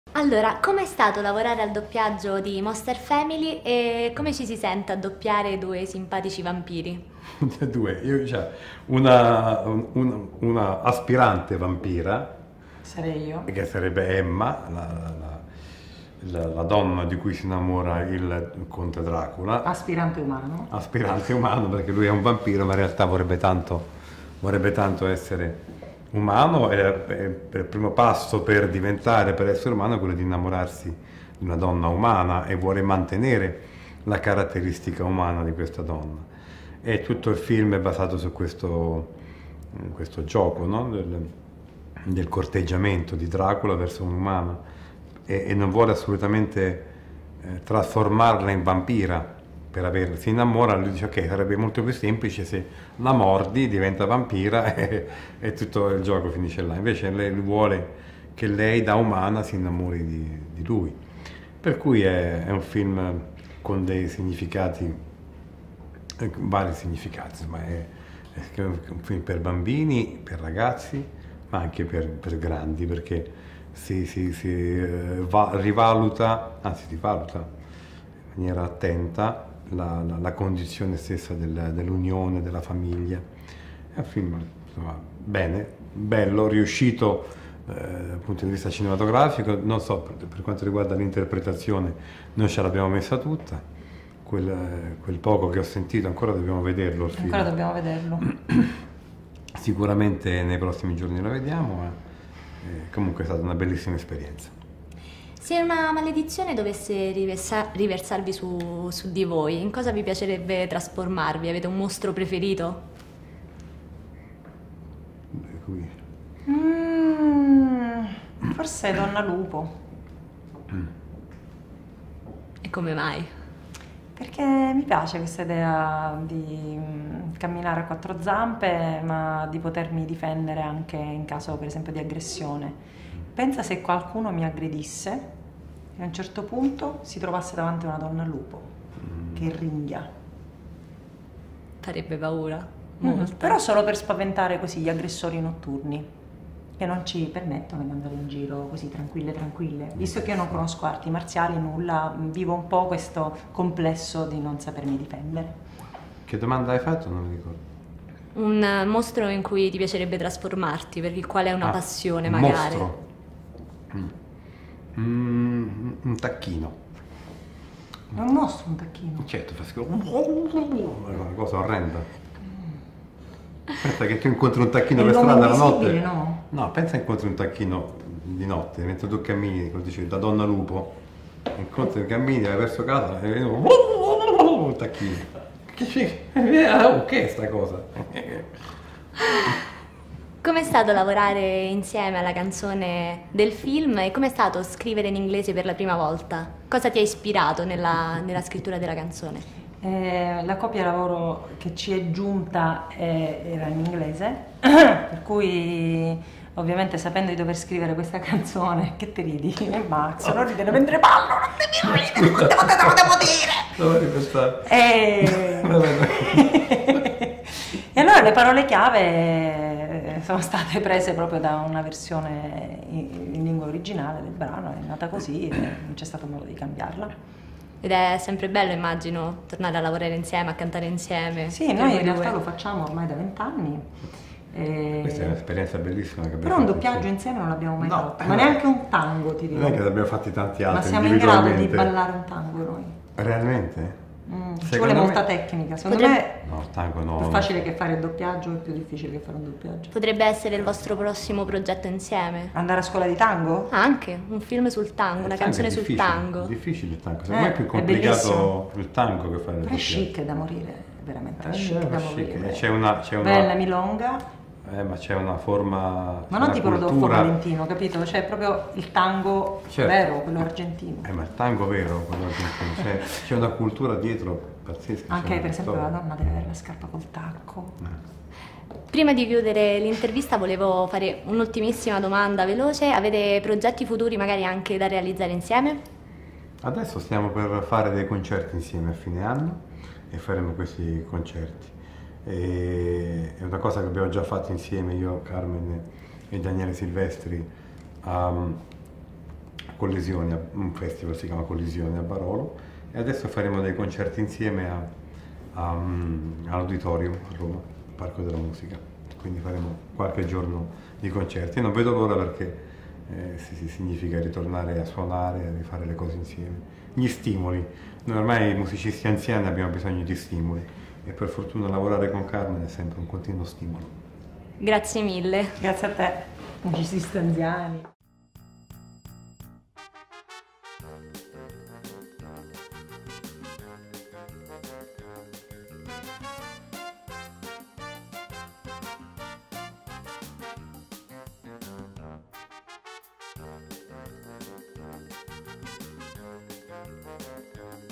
Monster Family | Intervista Esclusiva ai doppiatori italiani Carmen Consoli e Max Gazzè
Monster Family | Intervista Esclusiva a Carmen Consoli e Max Gazzè: Martina Barone intervista per Cinematographe - FilmIsNow i doppiatori italiani del film d'animazione Monster Family.